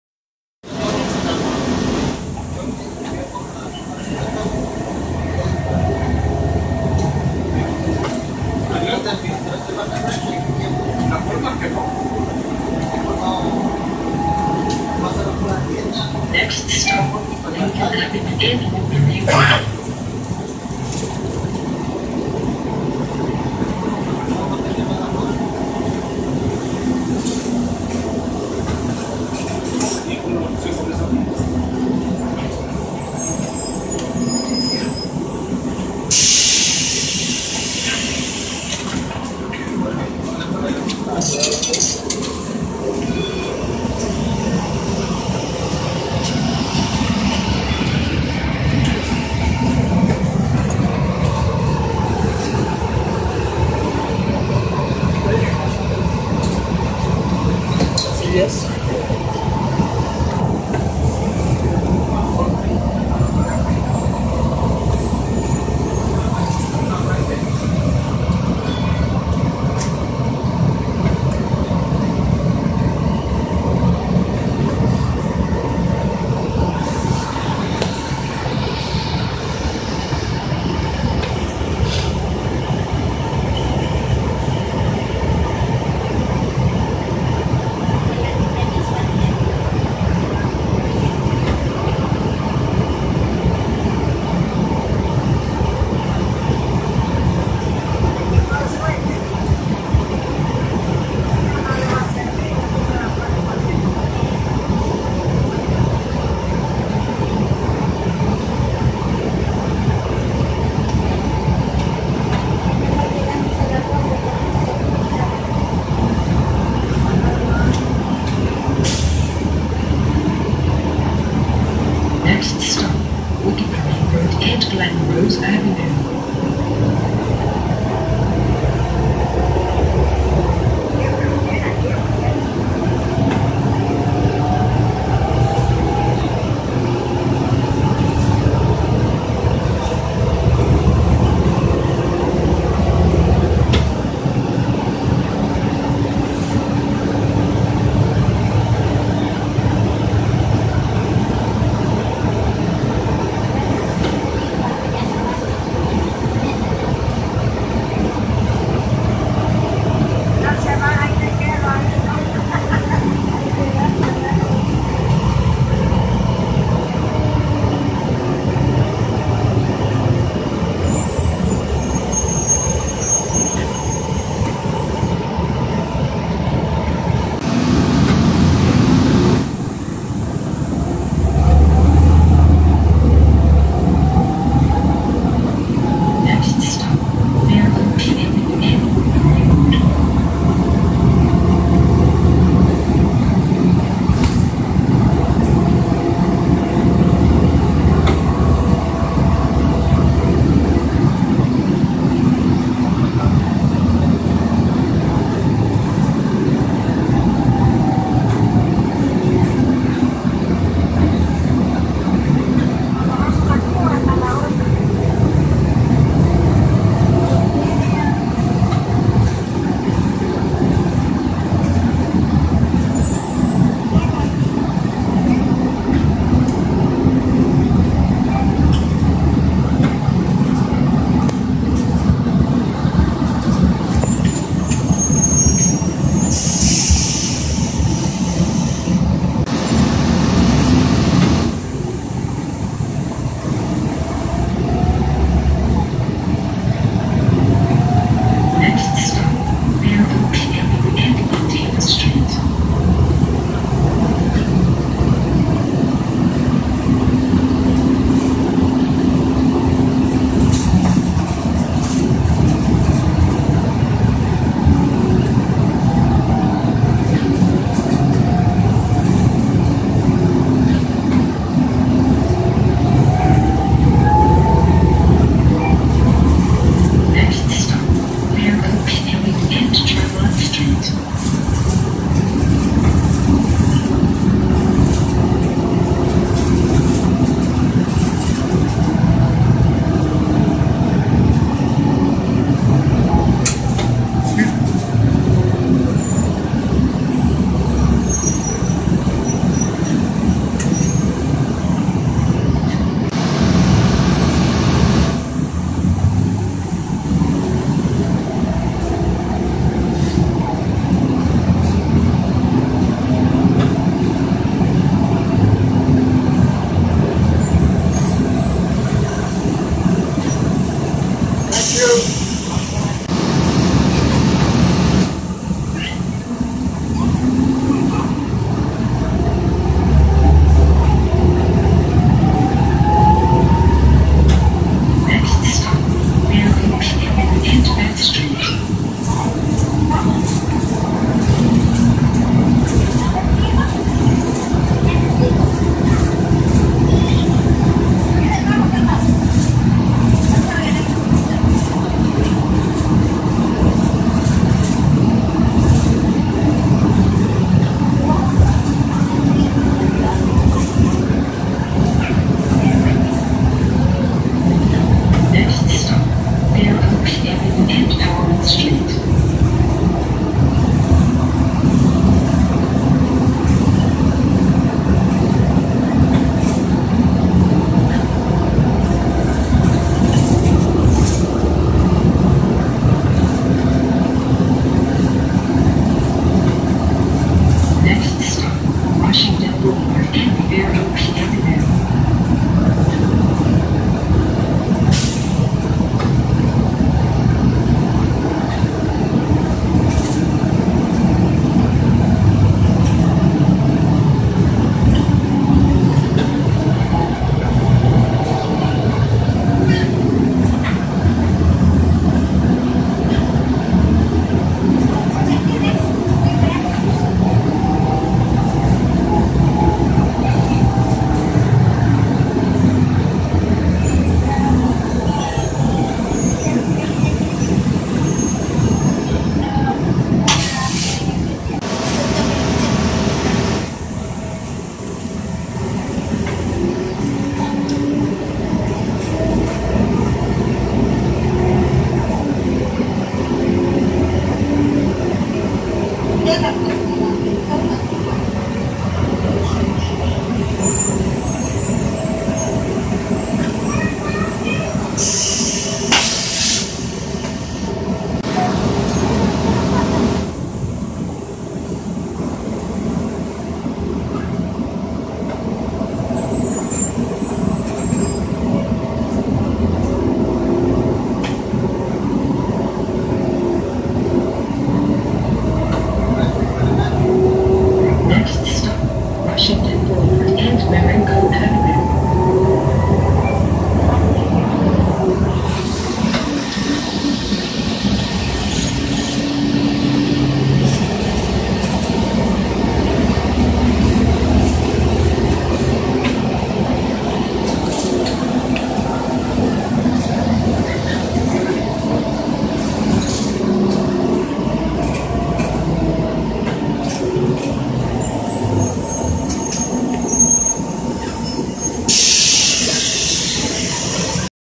2017 Pasadena Transit ENC EZ RIDER II BRT 3372
I rode 3372 on the 31. 3372 and all others were delivered on August 2017. On December 2017, 3372 started service.

Specs:
Cummins Westport ISL G
Allison B300R
Hanover Monochrome Amber

Date: 3/13/18
Edited with WeVideo

After Washingon / Fair Oaks, I boarded off at Marengo, so I accidently typed "Fair Oaks" the second time.